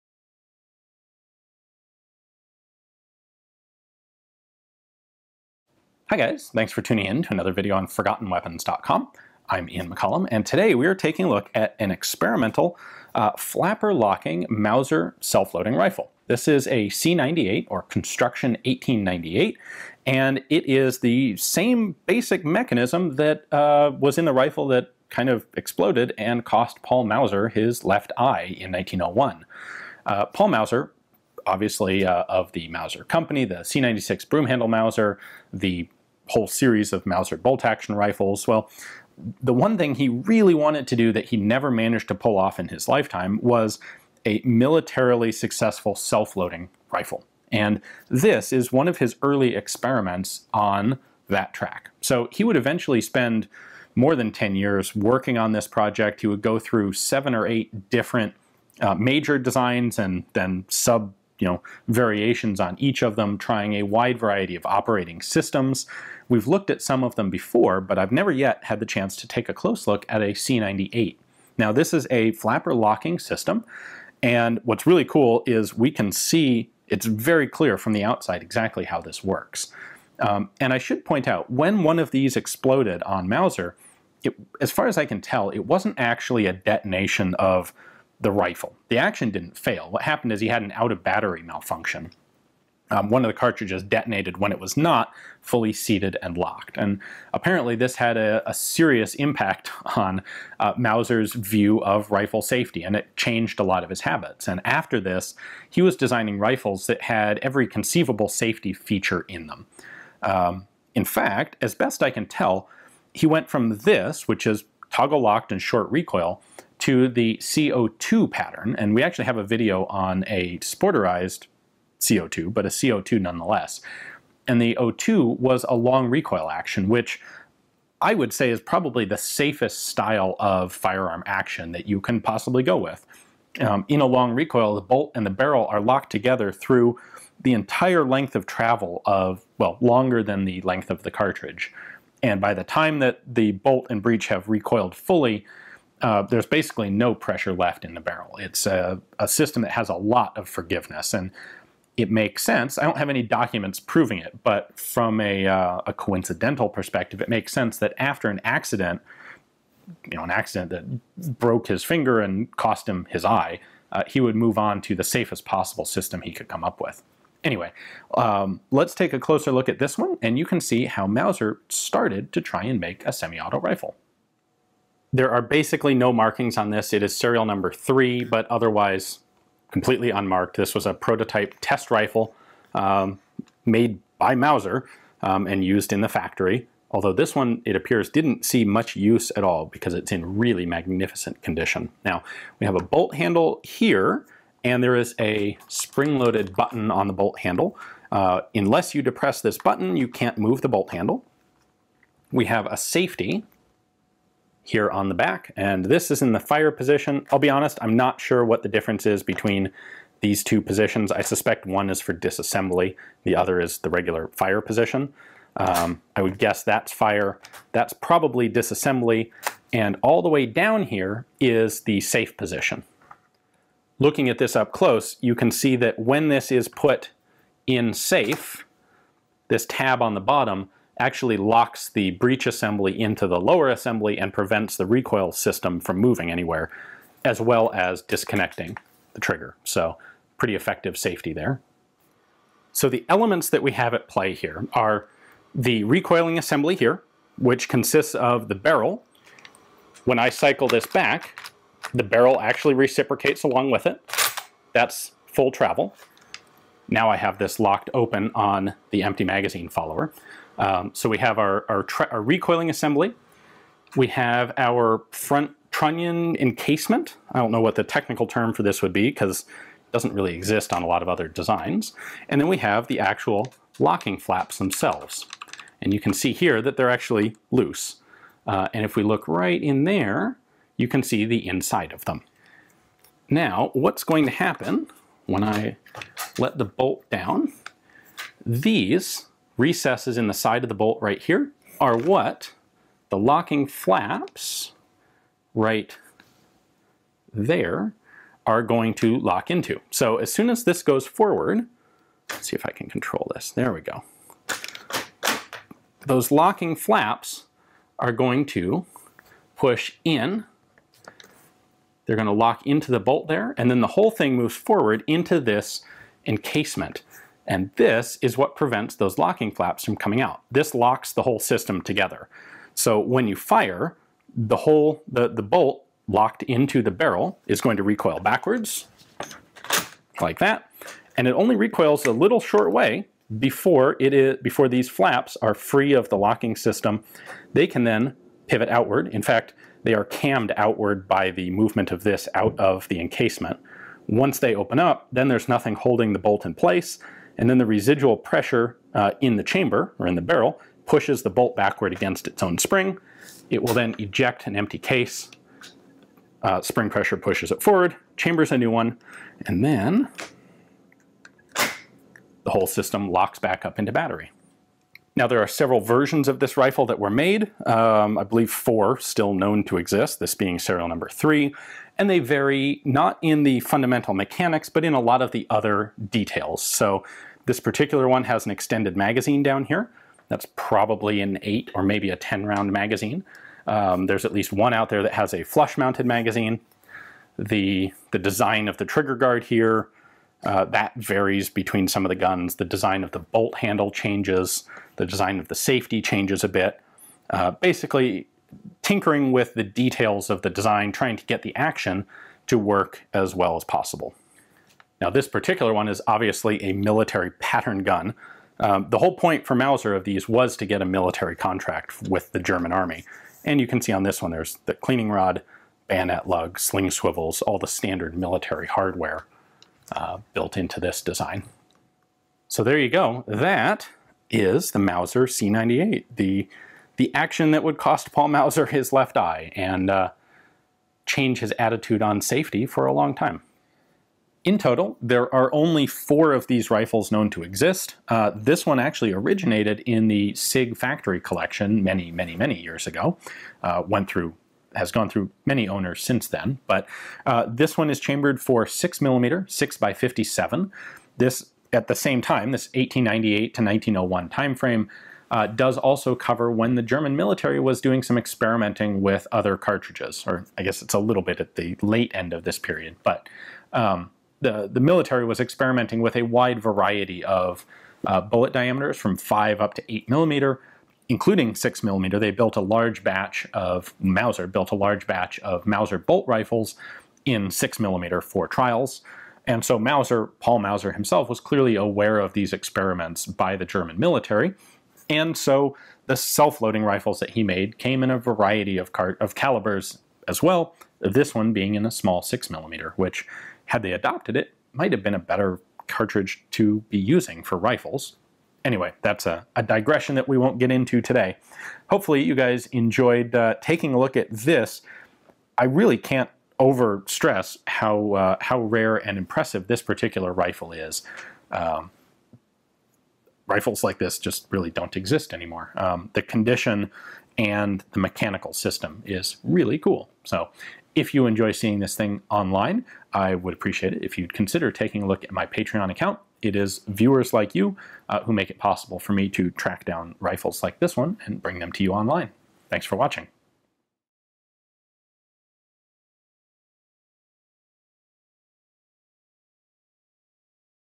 Mauser C98: The System That Cost Paul Mauser an Eye
One of Paul Mauser's lifelong projects was the design of a semiautomatic rifle for the German military. He would go through a multitude of different designs searching for something that would be sufficiently reliable, durable, and simple - and ultimately he would never fulfill the goal. But his efforts left us a trail of very interesting prototypes!

In 1898 he conceived this sort of flapper-locked system, on a short recoil action. It is actually a pretty clever system mechanically, but apparently lacked sufficient protection against an out of battery detonation. In 1901, one of the C98 rifles of this pattern (but not this specific gun) suffered an out of battery detonation while Mauser was firing it, and the explosion broke his finger and took his left eye. After this experience, Mauser would not give up on his search for a military selfloader, but his subsequent designs (like the long-recoil C02 pattern) would have a much greater emphasis on mechanical safety.